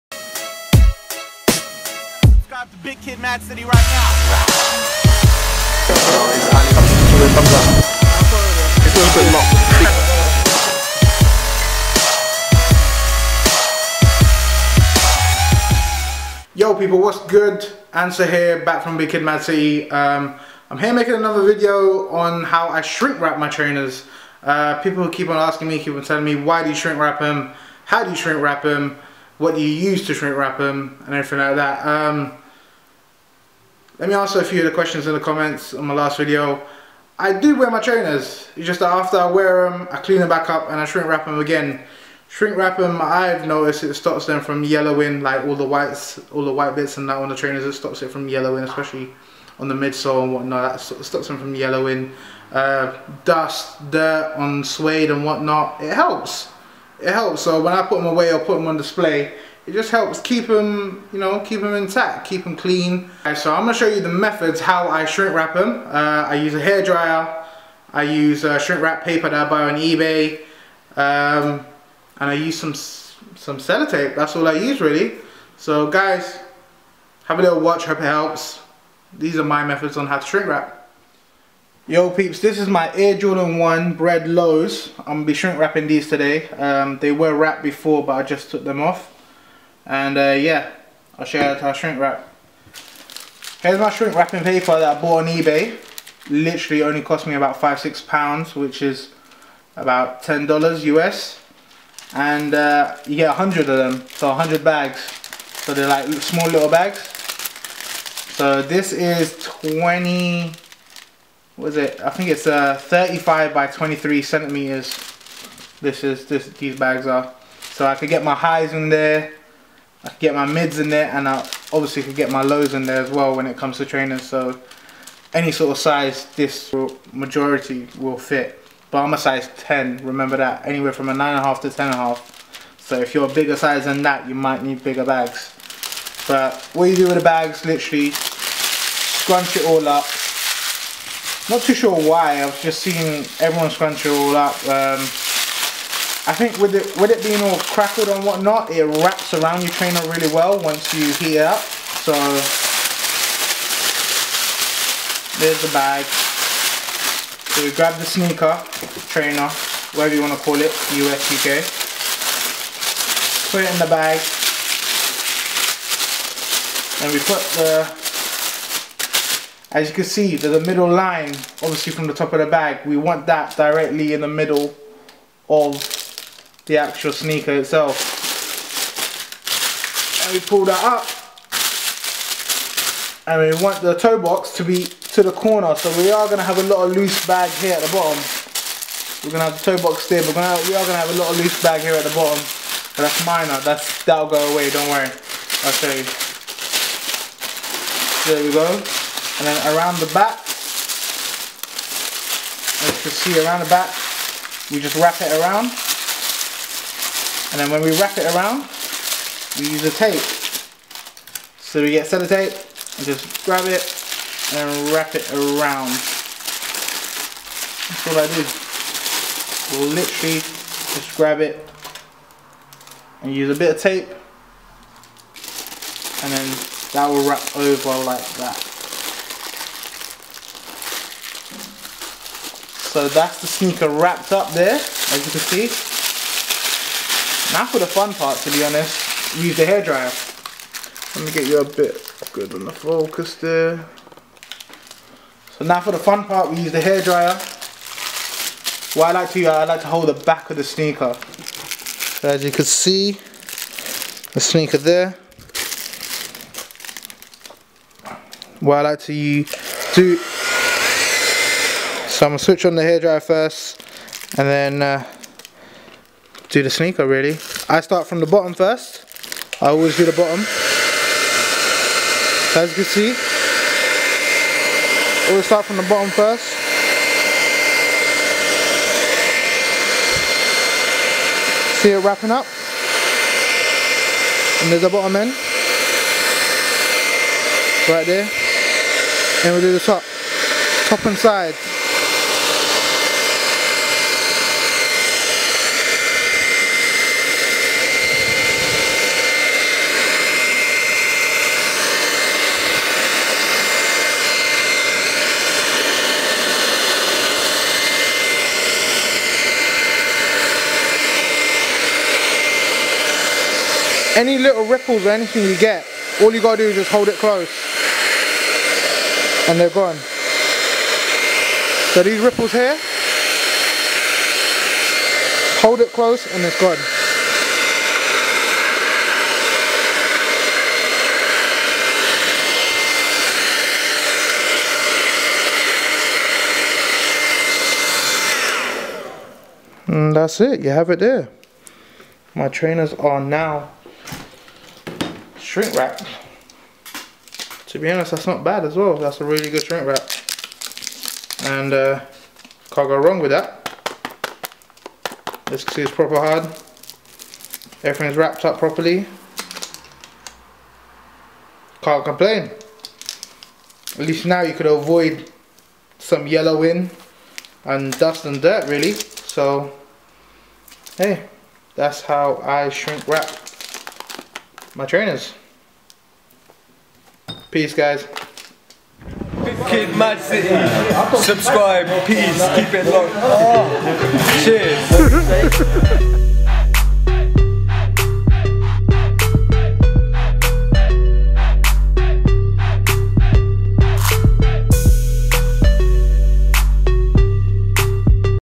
Shrink Wrap Sneakers!!! Jordan Bred 1 Low
My Tutorial on how to shrink wrap Sneakers
Nike AirJordan Bred 1Being Shrink Wraped